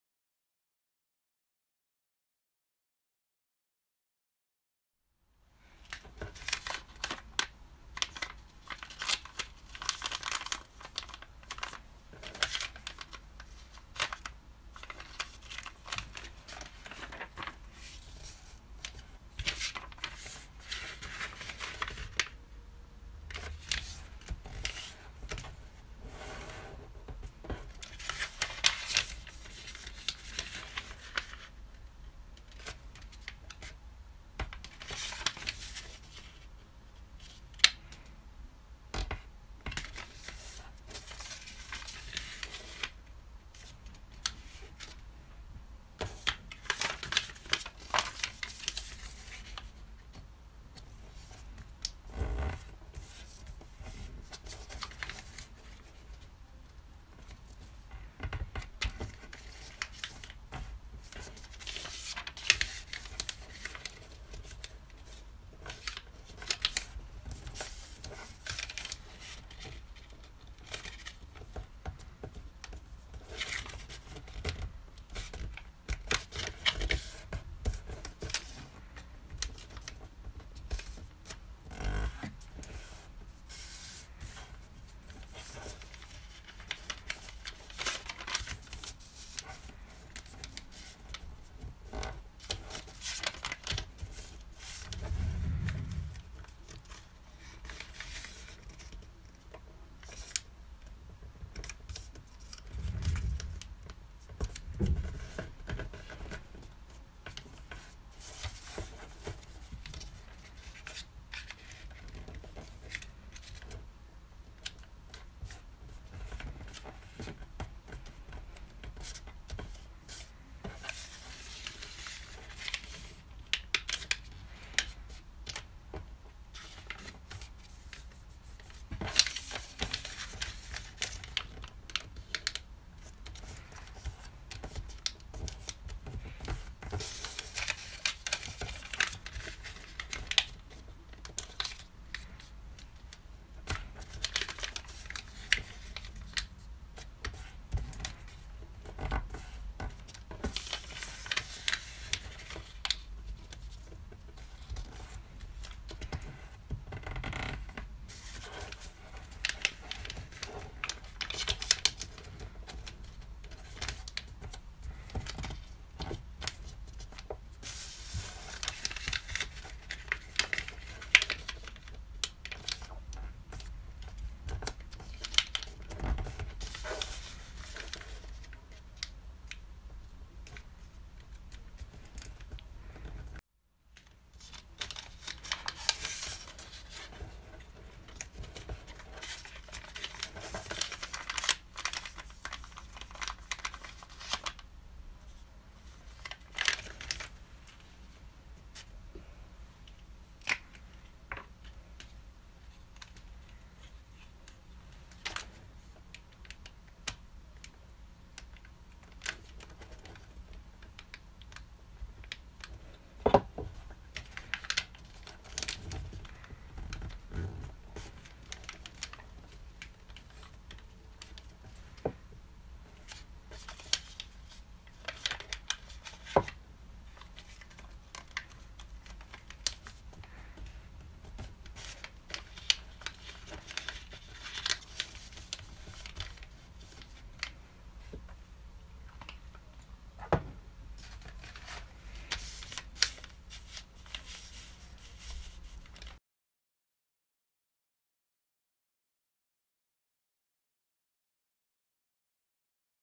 [색종이 접기]편지봉투접기
색종이 한장으로 하트모양 편지 봉투 접기